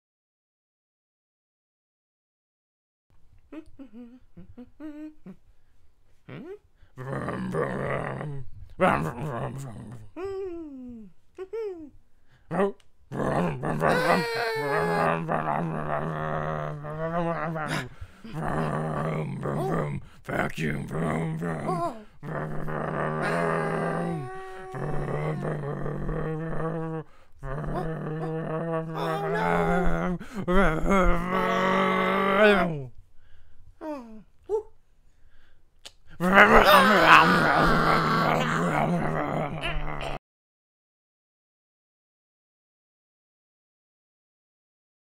Stop motion animation "Dust Bunny Vs. The Vacuum"
"Dust Bunny vs. The Vacuum"
  Project 2 Art-219

  Vacuum sound effects- Sam Cline